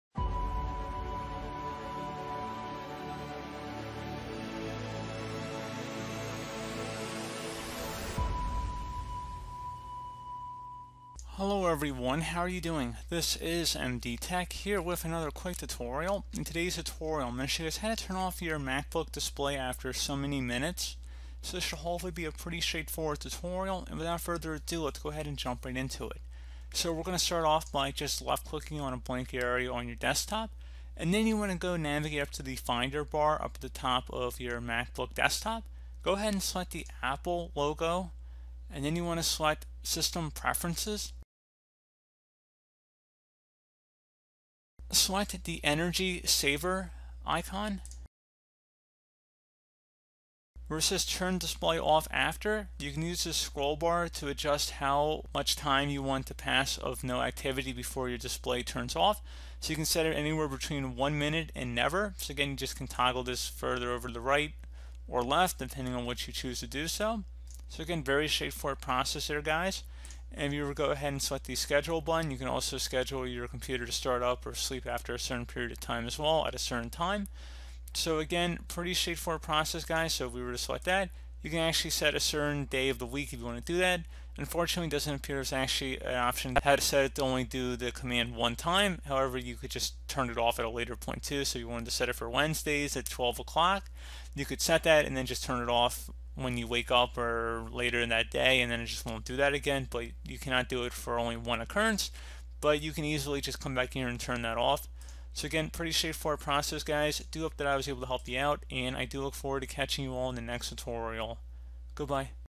How To Turn Off MacBook Display After So Many Minutes [Tutorial]
You can set your Mac to go to sleep after a specified duration of inactivity. You can also set your Mac to go to sleep and wake up at a specific time.

Schedule specific times for your Mac to start up, shut down, go to sleep, or wake from sleep. You can do this if you want to be sure your Mac turns off when you aren’t working and turns on before you come to work.